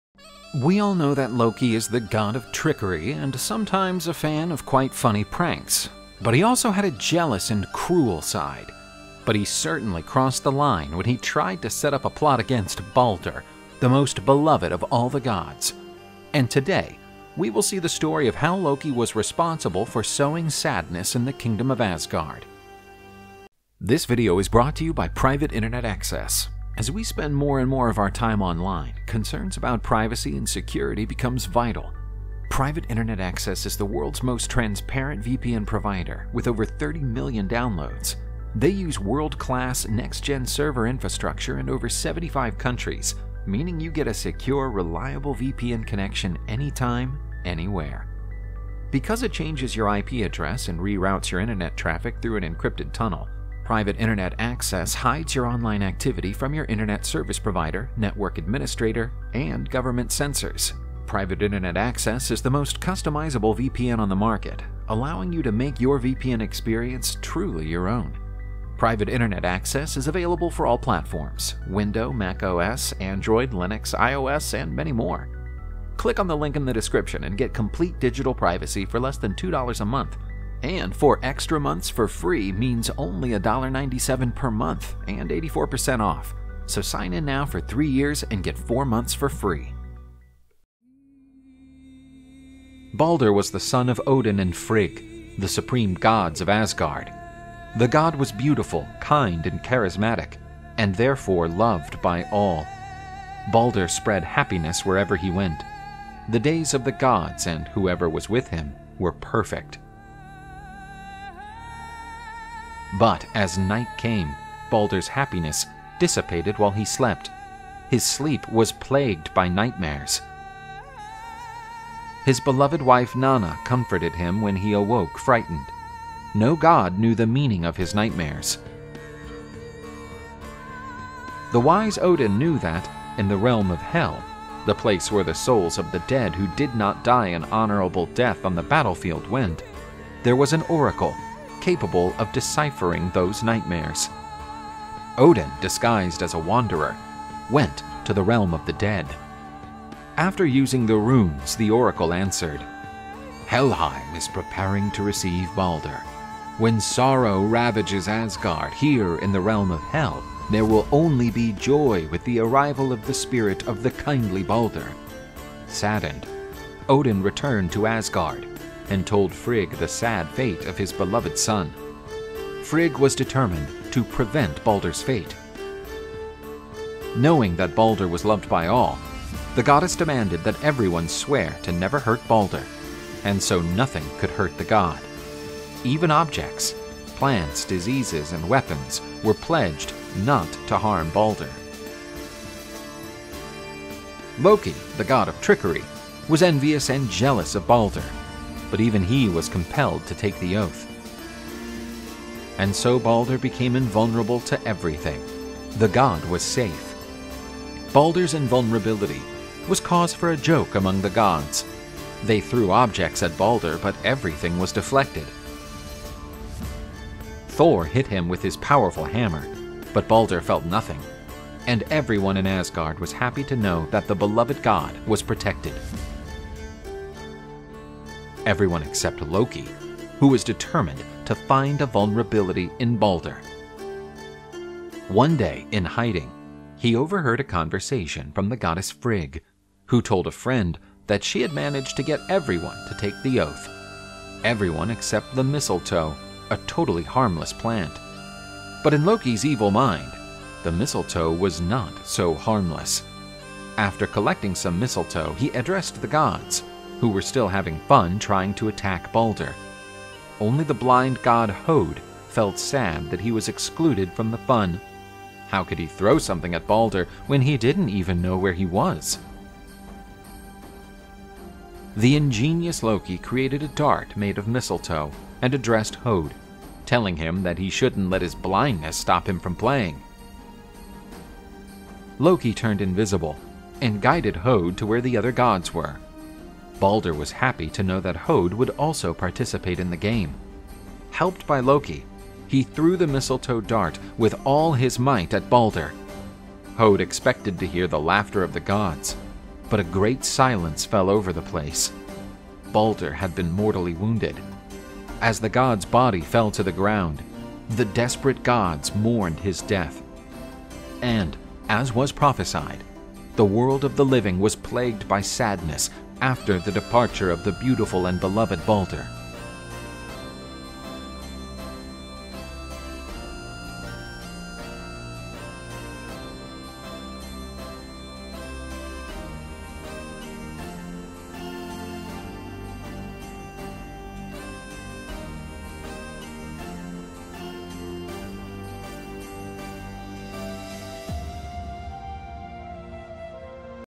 Loki and the Sad Fate of Baldr( Balder/Baldur) the Most Beloved of the Gods - Norse Mythology
Art: Mauro Fodra
Color: Rod Fernandes
Script: Bruno Viriato